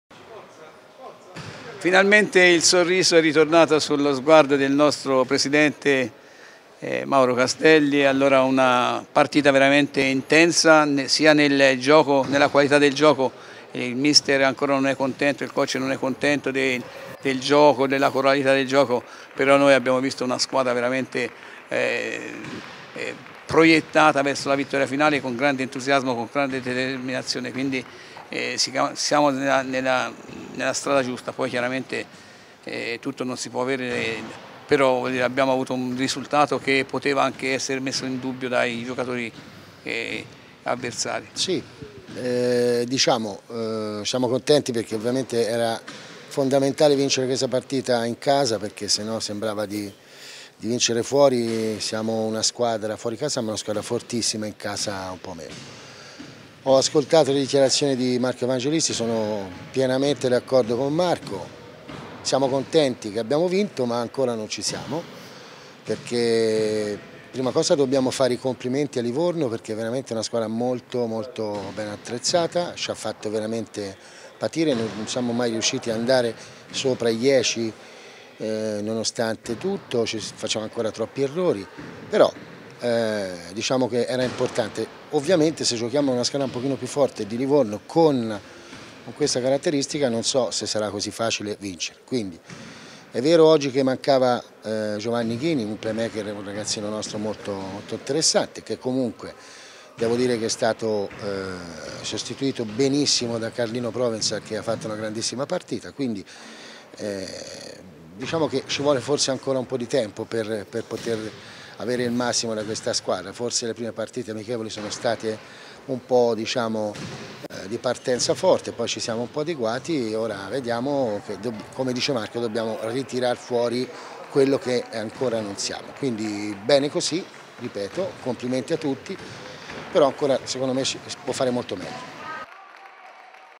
Successo importante in casa per la Scuola Basket Arezzo
Successo importante per l'Amen Scuola Basket Arezzo che trionfa in casa con il Toscana Food Don Bosco Livorno al termine di una gara combattuta.

Ottima partenza per gli amaranto che chiudono il primo quarto con ben 27 punti realizzati. Buona comunque la gara del Don Bosco che ha la capacità di rimanere sempre dentro la partita.  A chiudere la gara sono i 5 punti in fila di Rossi che permettono agli amaranto di conquistare i due punti con l'Amen che chiude 72-62. Migliori realizzatori della serata la coppia Brandini-Rossi che ha guidato l'attacco aretino in questa ottima vittoria in casa.